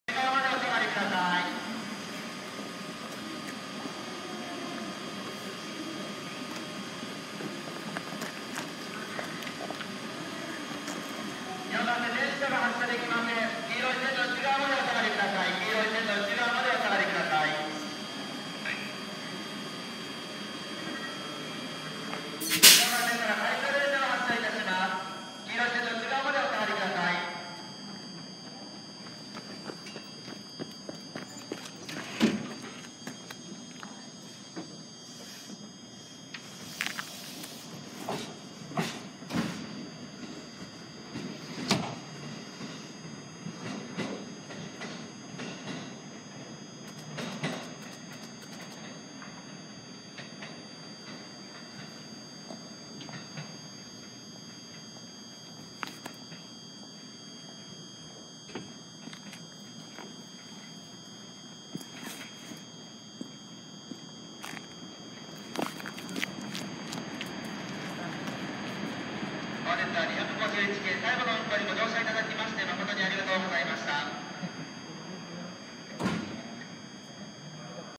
【251系ラストラン】運用終了後の小田原駅発車《ホームライナー小田原23号》3/13(ダイヤ改正前日)
2020/03/13 に撮影

翌日3/14にダイヤ改正があり、E261系のサフィール踊り子号がデビューする一方で、251系のスーパービュー踊り子号は引退することとなりました。
撮影時は、既にスーパービュー踊り子としての運用は全て終了していましたが、251系の運用は、朝晩のライナー列車でも僅かに存在しており、251系の本当のラストランは、私がここまで乗車してきたホームライナー小田原23号となりました。
私はライナー発車の2時間前には新宿駅に行きましたが、既に券売機の前には大行列。
それでも最終的には、私の前よりも遥かに多い人数の方が、私の後ろに並ばれており、251系の人気の高さを感じました。

ちなみによろしければ、251系のラストランの車窓を全区間で撮影したのでご覧頂けると嬉しいです♪